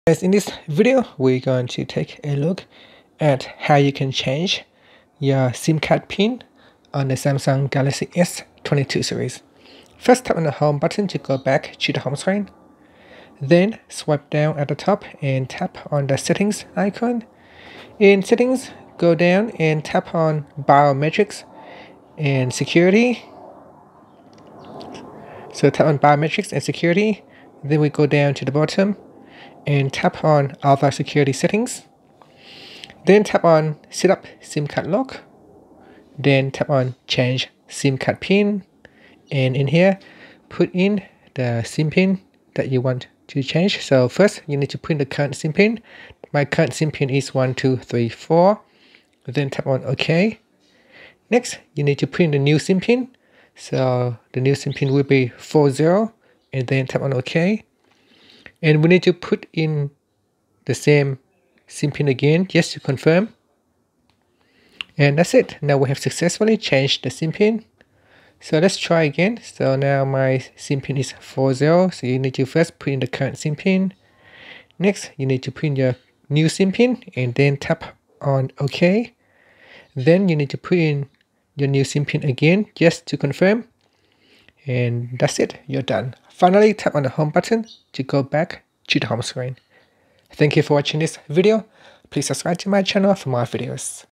Galaxy S22/S22+/Ultra: How to Change SIM Card Pin
Learn how you can change the SIM card pin on the Samsung Galaxy S22 / S22+ / S22 Ultra.